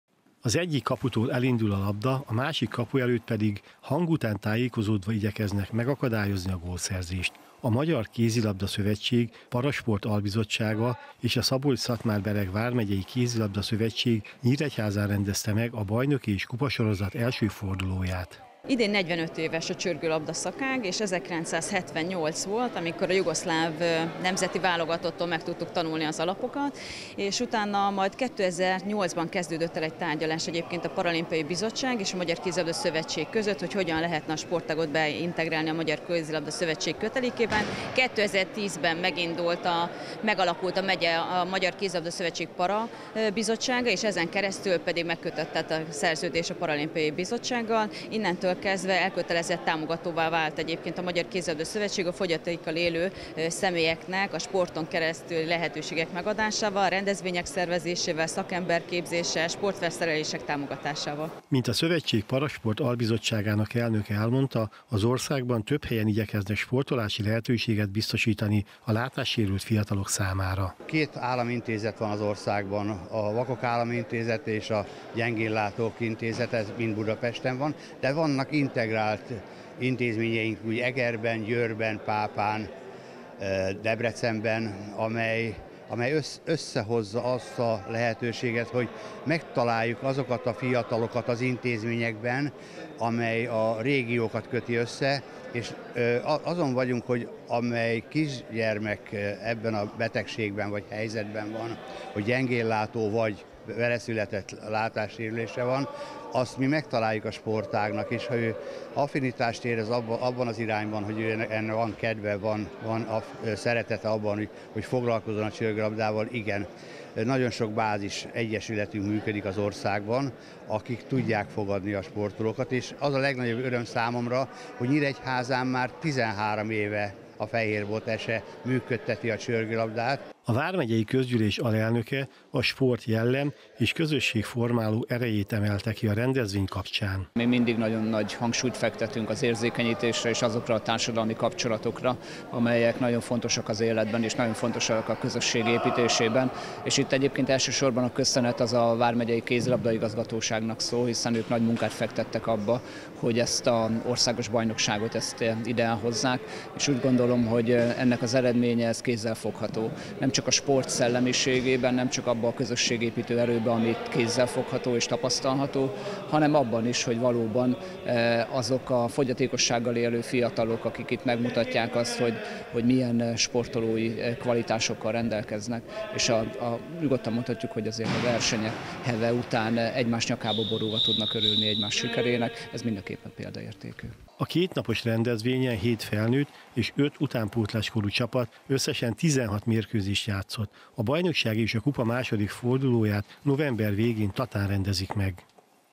Csörgőlabda bajnokság
Hét felnőtt és öt utánpótlás csapat mérte össze tudását Nyíregyházán a Csörgőlabda Bajnokság és Kupa első fordulójában. A Zrínyi Gimnázium tornatermében összesen tizenhat mérkőzést játszottak a csapatok.